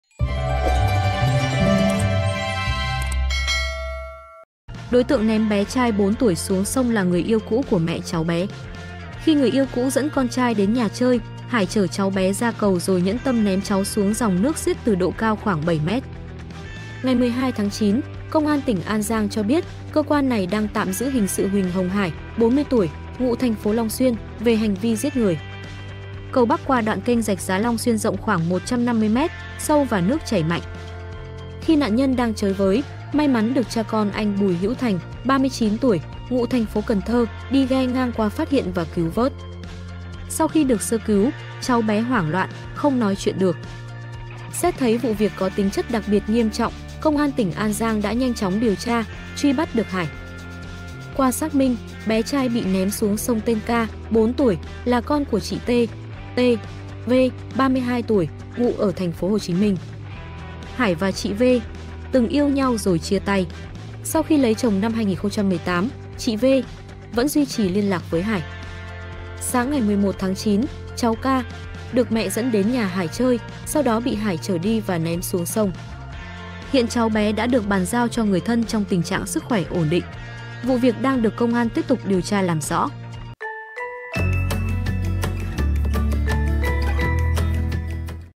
Đối tượng ném bé trai 4 tuổi xuống sông là người yêu cũ của mẹ cháu bé
Khi người yêu cũ dẫn con trai đến nhà chơi, Hải chở cháu bé ra cầu rồi nhẫn tâm ném cháu xuống dòng nước xiết từ độ cao khoảng 7m.